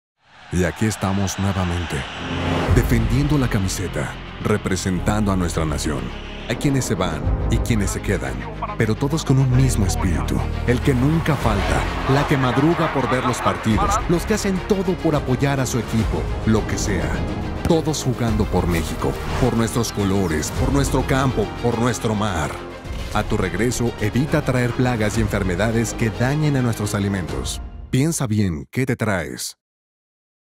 Rumbo a Rusia 2018 ¡Ponte la Camiseta! | SENASICA
En este Mundial de Rusia 2018, se parte de nuestra selección y no le metas gol a México.
¡Viaja sin plagas!